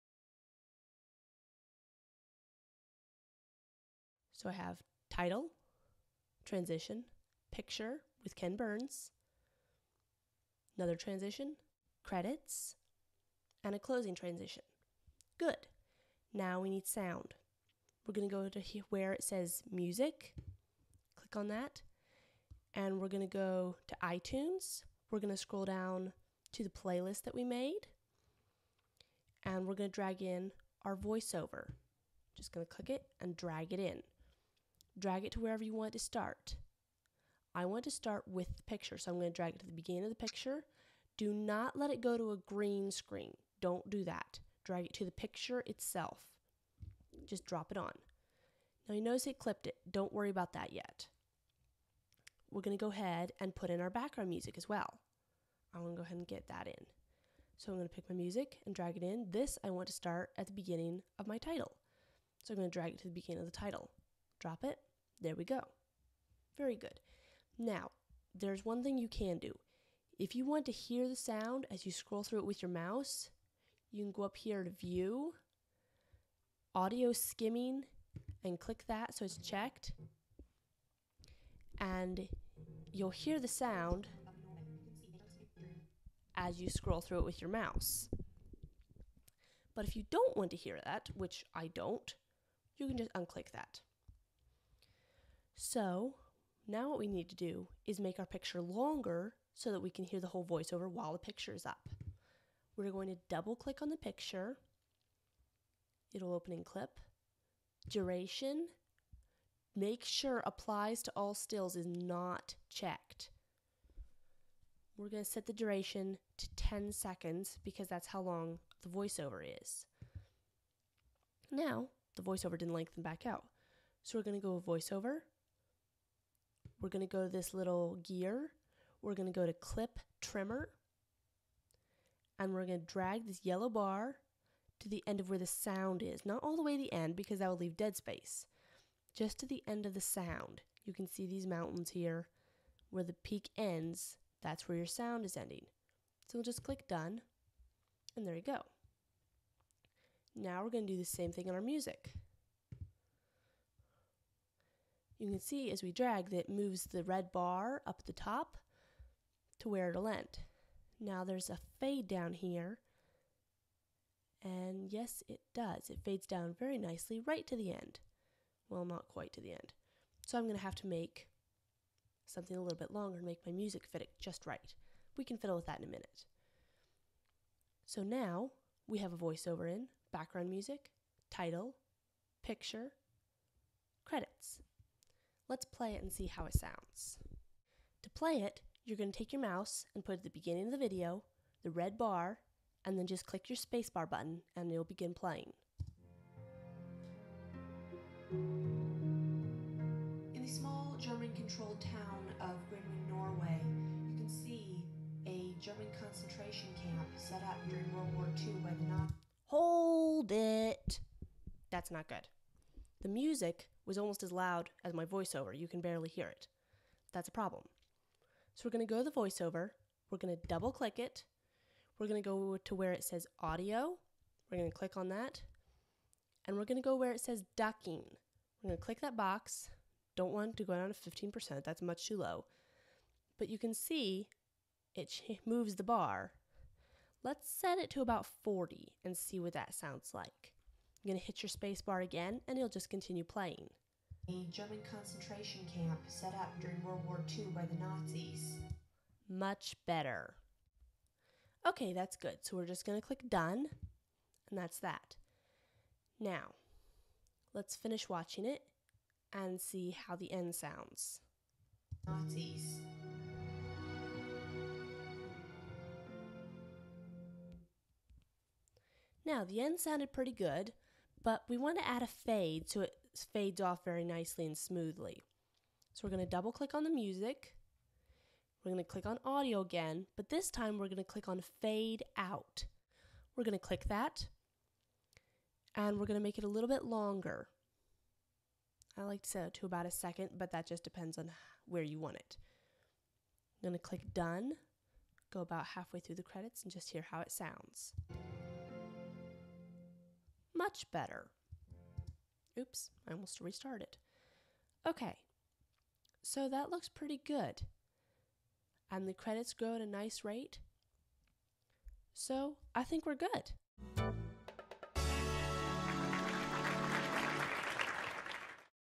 How to Add Sound to iMovie
The fourth part in my series on making a documentary in iMovie.  Covers adding and syncing sound.